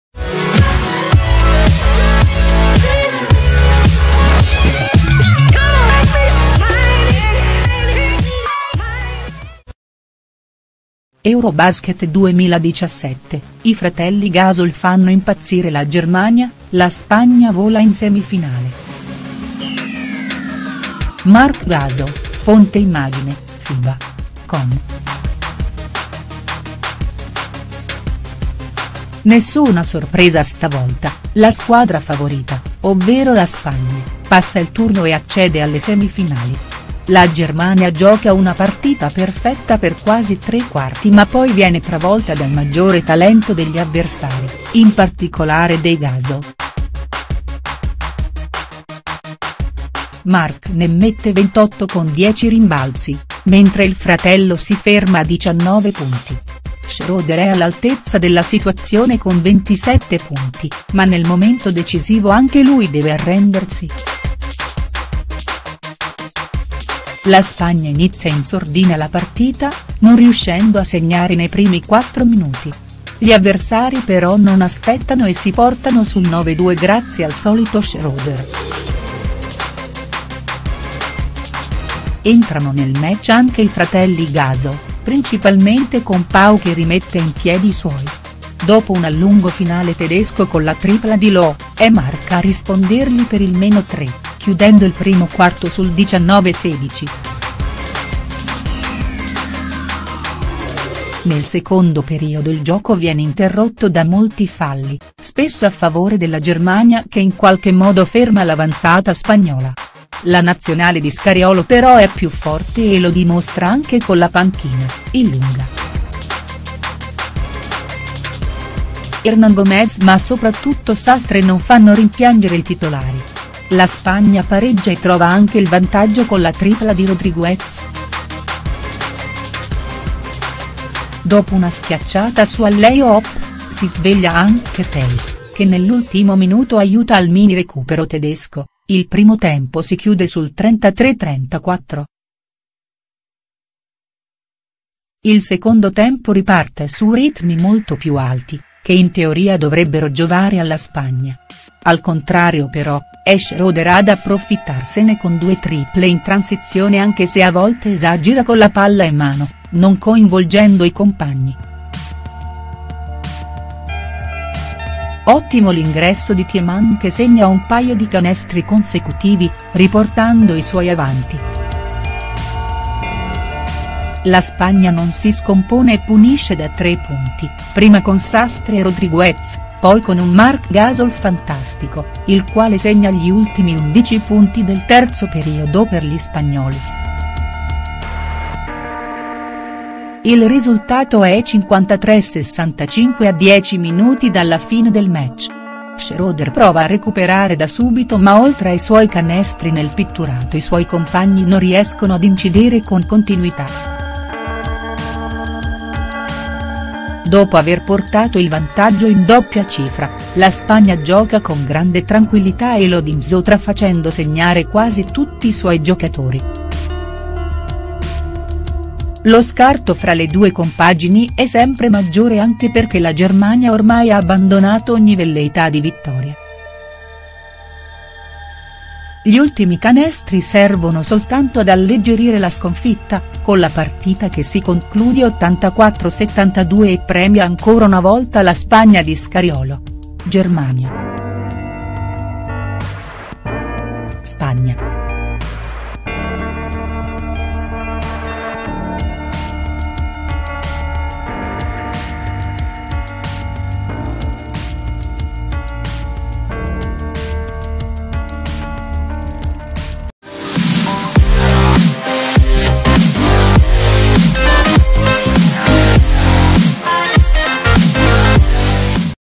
Eurobasket 2017 - I fratelli Gasol fanno impazzire la Germania: la Spagna vola in semifinale.HD
Dopo un primo tempo combattuto, Marc e Pau chiudono il match dimostrando grande personalità. Niente da fare per Schroder nonostante la fantastica partita da 27 punti.